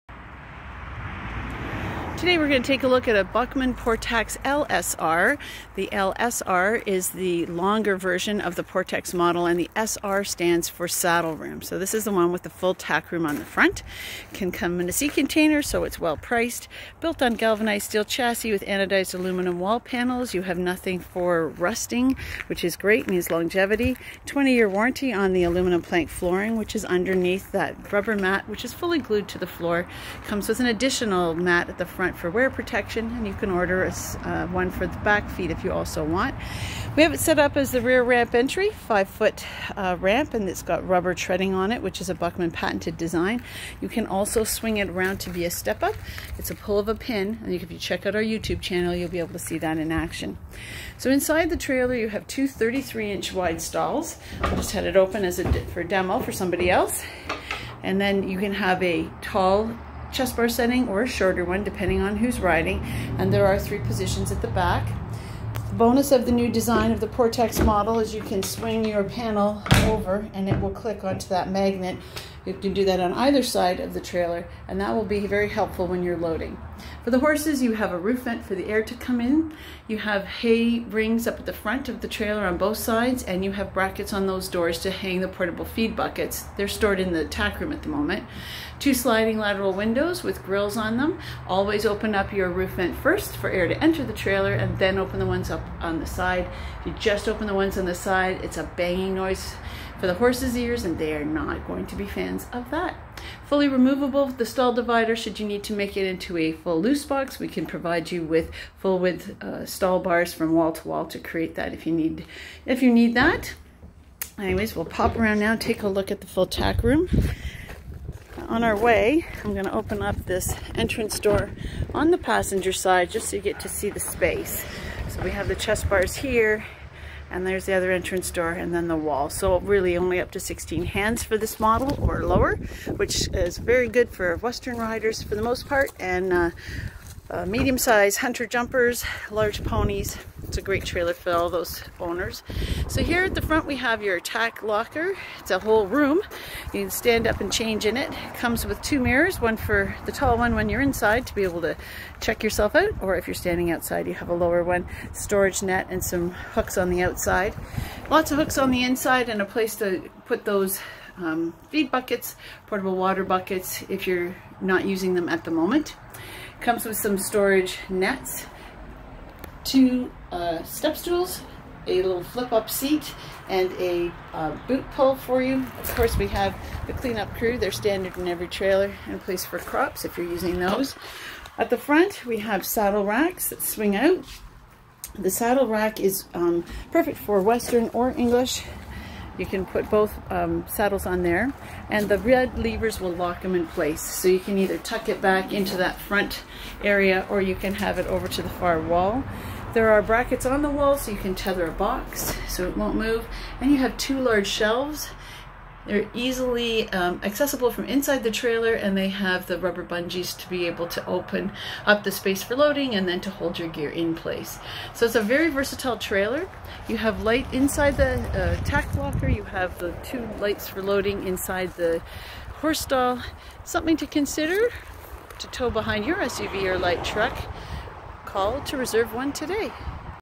2024 Portax LSR
Böckmann's Portax LSR - ideal for the owner of horses 16hh or less! Do you have Medium horses, QH's, Tennessee Walkers, Arabians, Paso Fino's or other medium size breeds? This trailer model may suit you!

The Portax LSR offers many conveniences. The full size tack room offers 2 large shelves with securing straps; brackets so you can secure upright tack boxes or hay and 2 saddle racks that can be locked into position for travel. Several hooks are available plus storage nets. A seat and boot pull that can be closed against the wall when not in use. Storage hangers for the portable feed/water buckets, when not in use, and plenty of floor space. Add sliding windows  on the side walls, or towards the horses for a surcharge.

The horse area has the Portax Central divider and swinging panels, with magnet panel holders, for loading/unloading. Two 33" wide stalls with 7'7" internal height, the stalls can easily accommodate horses up to an 84" blanket- BUT the head space is for the medium (to 16hh) horse and smaller!  For larger horses AND a tack room- ask us about the Big Portax.

An added rubber mat for wear protection against pawing is a standard feature. The full floor has a rubber mat glued to the aluminum plank flooring underneath and sealed at the walls. That plank flooring has a 20 year Warranty!!  Adjustable rear and chest bars, allow you to create the safest space for your horses.  Hay rings, trailer tie loops, air vent in the roof, double sliding lateral windows in each wall, and the adjustable rear storm canvas with integrated wind screen keep the horse stall temperate.

LED Lighting in both the horse stall and the tack room is controlled by switches in the tack room.

Choose the colour of the roof/front bow! Choose your Decor! Add an external Tie Bar/saddle rack or Mounting step - make the Portax LSR uniquely yours!